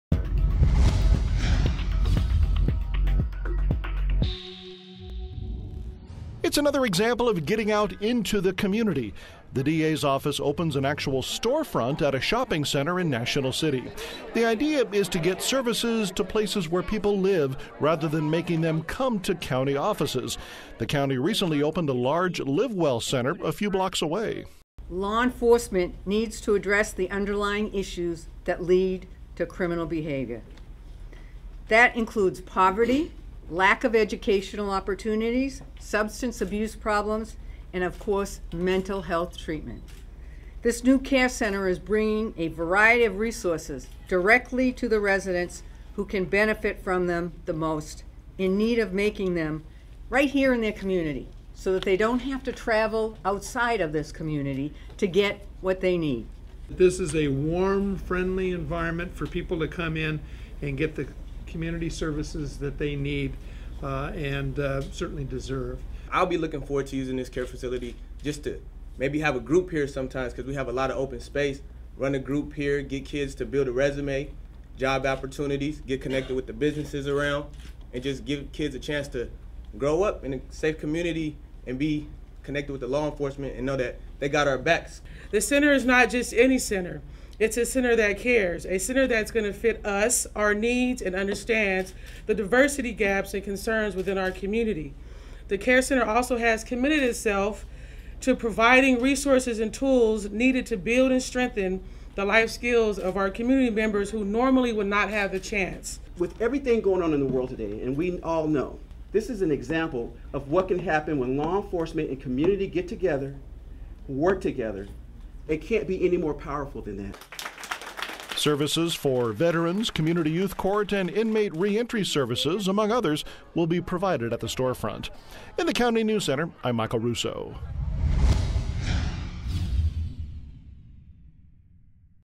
DA Partners with Southeast San Diego to Bring Services Directly to the Community
San Diego County District Attorney Bonnie Dumanis in conjunction with community partners from southeast San Diego and county and city dignitaries announced today the opening of the Community Action Resource Engagement Center (CARE) in National City. The CARE center is a product of community collaboration aimed at preventing crime and improving the quality of life of individuals living in Southeast San Diego. Over the last 12 years, the DA’s Office has focused on issues related to crime prevention, community intervention, and prisoner reentry. The CARE Center is an outgrowth of that commitment.